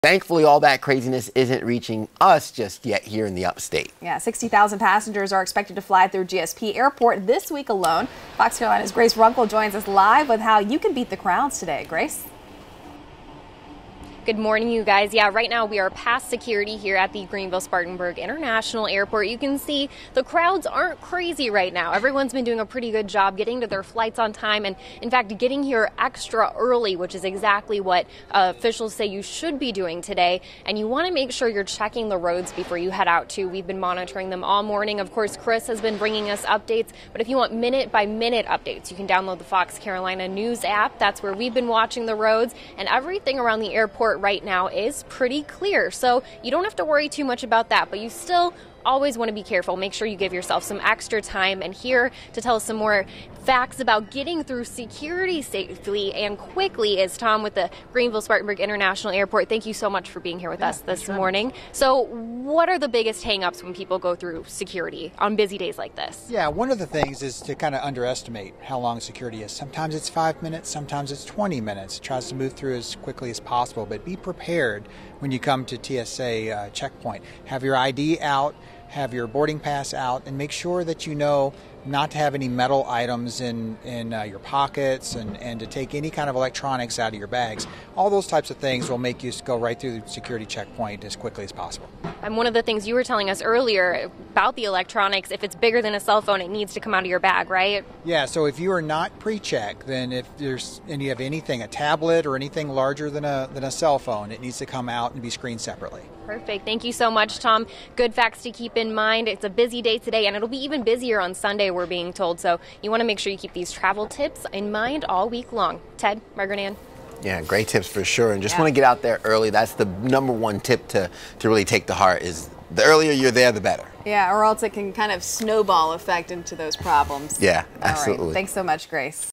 GSP expecting thousands for holiday travel
With 2.2M South Carolinians expected to travel, thousands are expected to head through GSP.  Grace has a preview.